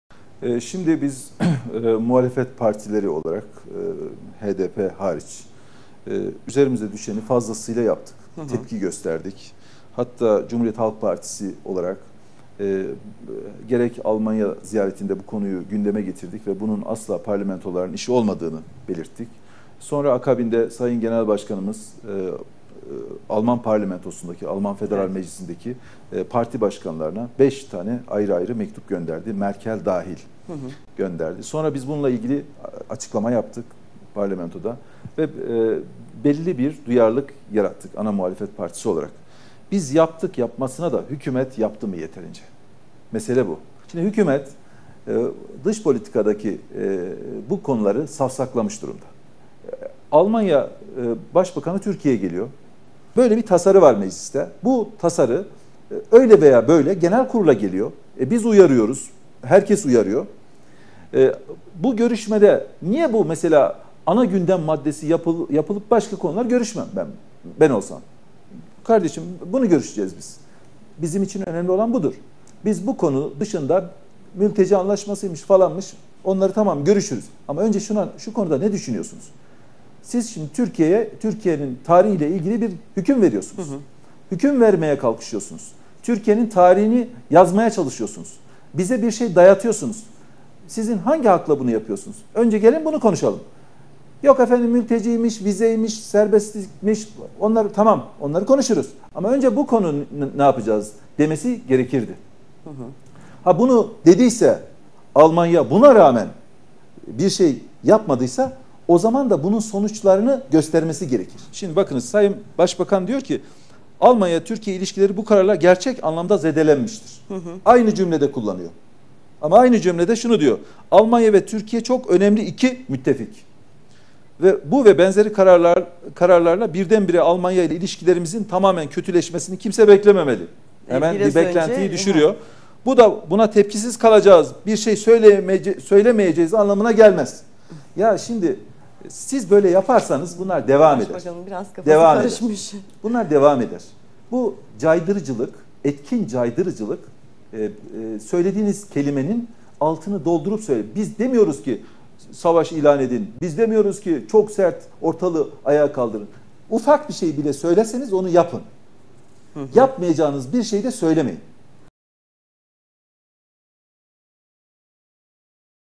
Hükümet Söylemlerinin Altını Doldurmalı!
Genel Başkan Yardımcımız Öztürk Yılmaz: Biz, sözde soykırım konusunda üstümüze düşeni yaptık. Peki hükümet, Merkel Türkiye'ye geldiğinde yapılan toplantıda, bu tasarıyı neden ana gündem maddesi yapmadı?